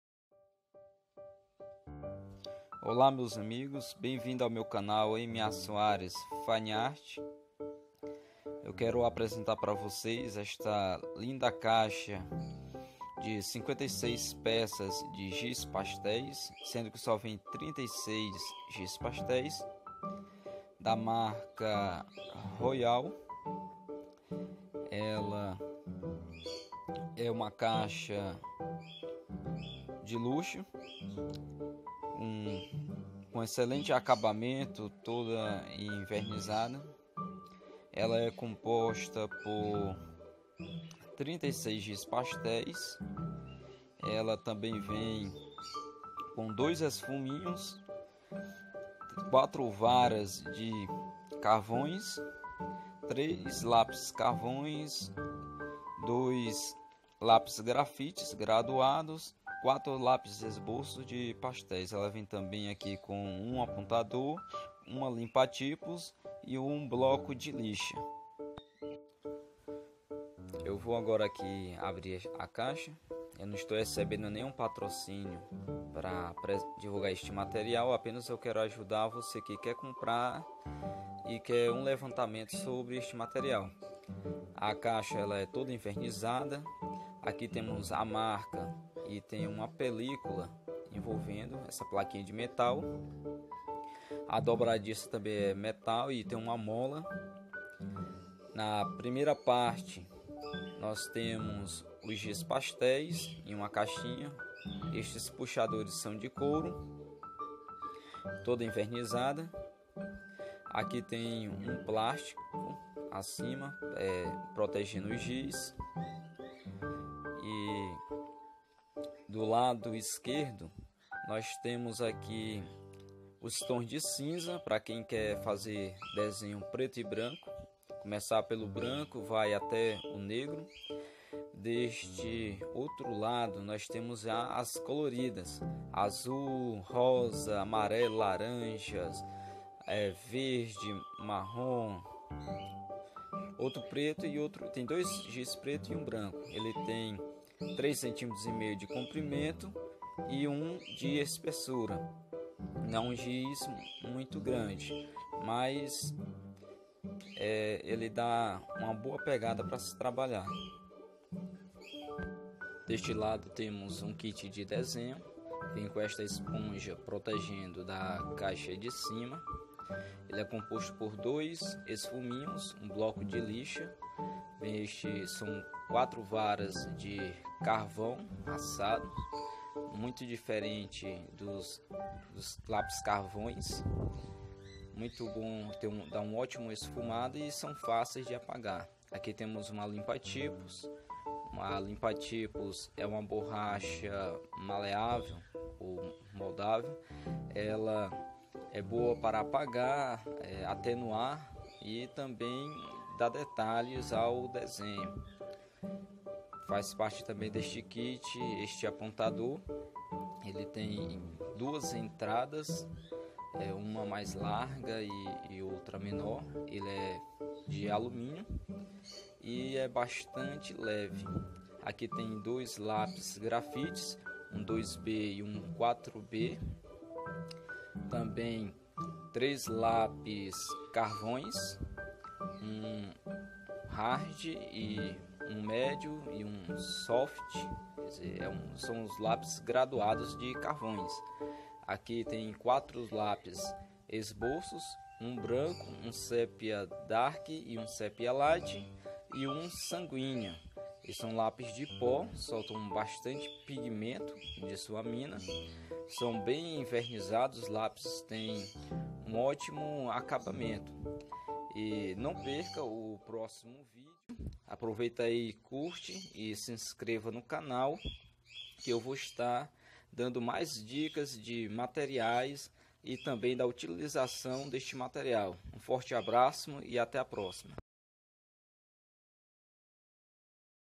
Giz pastel seco, lápis grafite, lápis carvão, esfuminho, apontador, Royal e Langnickel.
Maleta envernizada, contendo 56 peças.